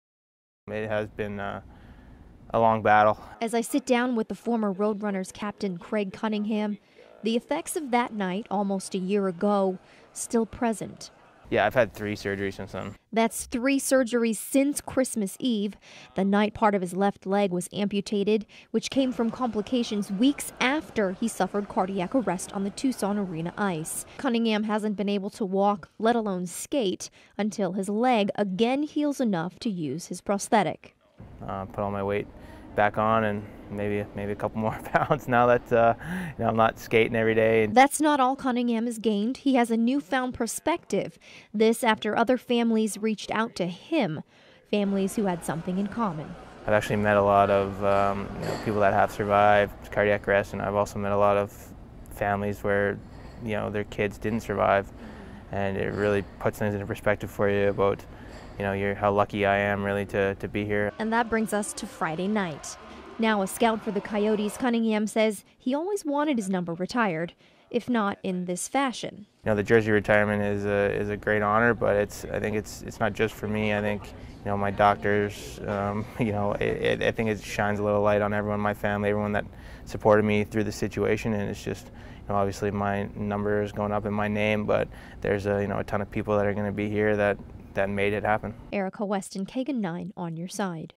Cunningham reflects almost a year later, ahead of jersey retirement
“It has been a long battle,” says the former Roadrunners captain.  As I sit down with Craig Cunningham, the effects of that night almost a year ago, still present.  “I’ve had three surgeries since then,” he tells me.